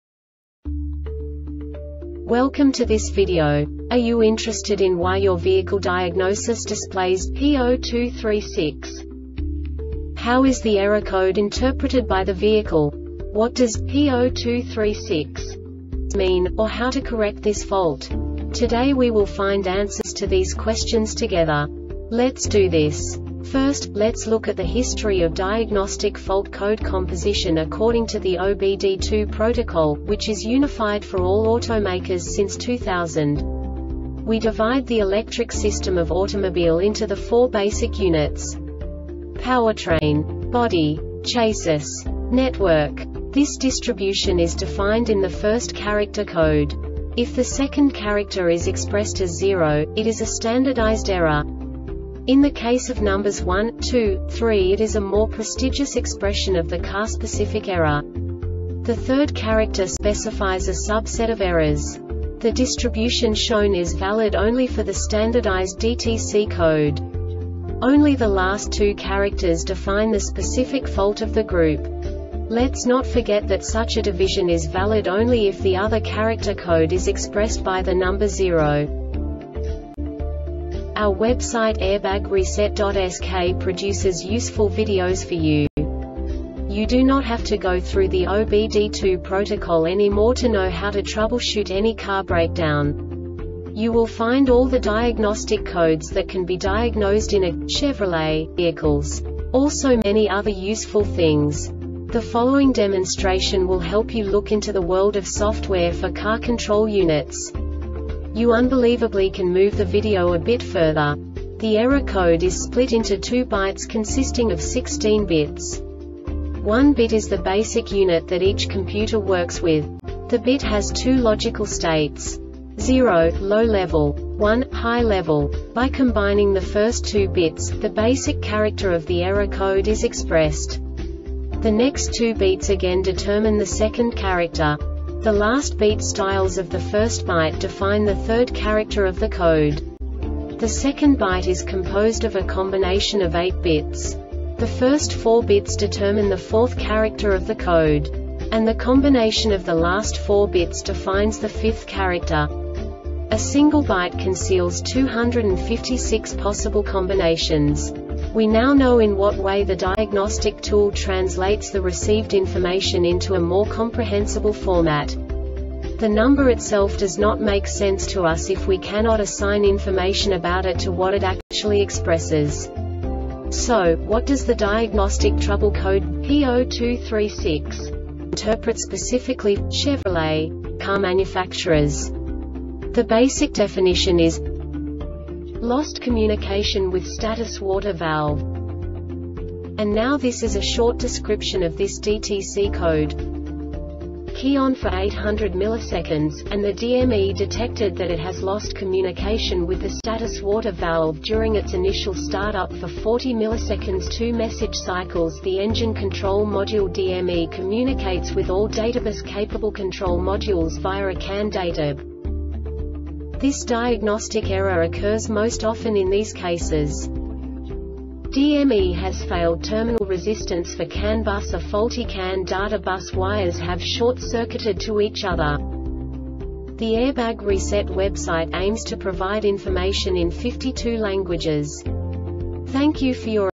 DTC Chevrolet P0236 Short Explanation
Contents:
0:21 Basic DTC analysis according to OBD2 protocol standard.
1:48 Insight into programming
2:58 A diagnostically understandable description of the Diagnostic Trouble Code
3:05 Basic error definition

Make:
Chevrolet
Code:
P0236
Definition:
Lost Communication With Status Water Valve
Description:
Key on for 800ms,and the DME detected that it has lost communication with the status water valve during its initial startup for 40ms (2 message cycles).The Engine Control Module (DME) communicates with all databus-capable control modules via a CAN datab
Cause: